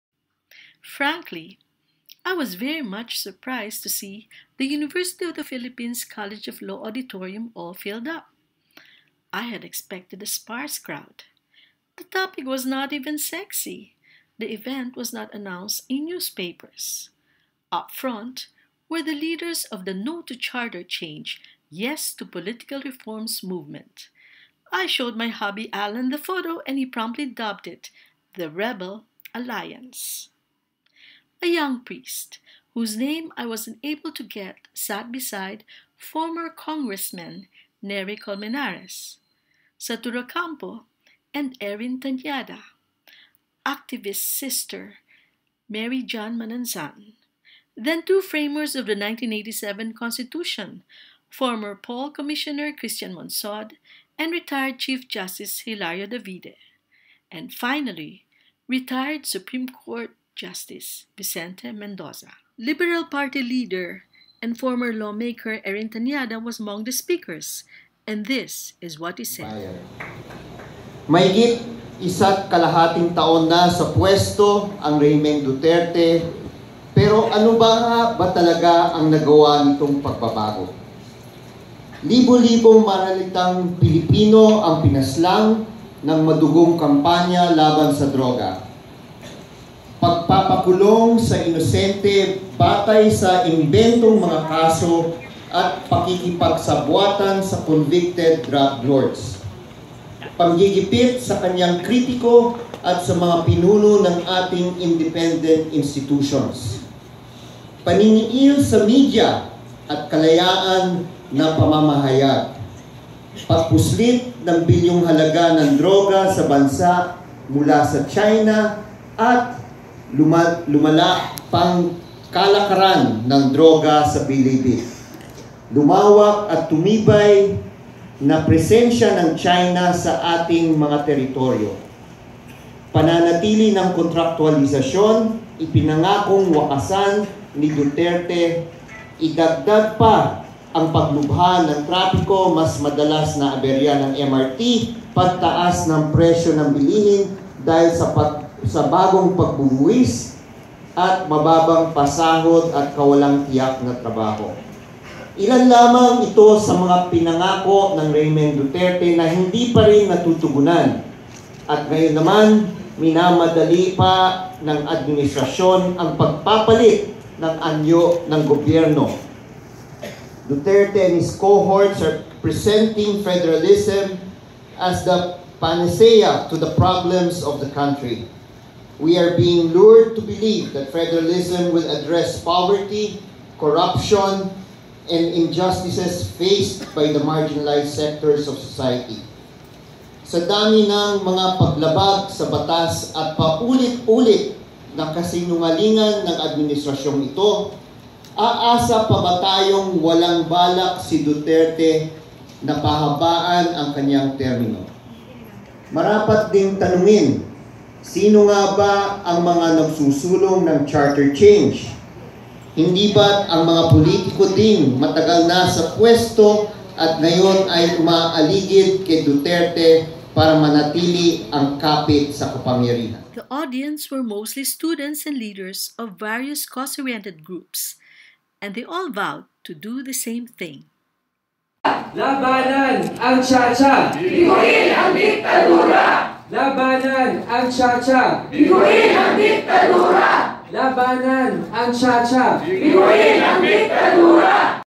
Movement against charter change takes off
A movement to stop President Rodrigo Duterte from replacing the 1987 Constitution with one that will give him awesome powers is underway.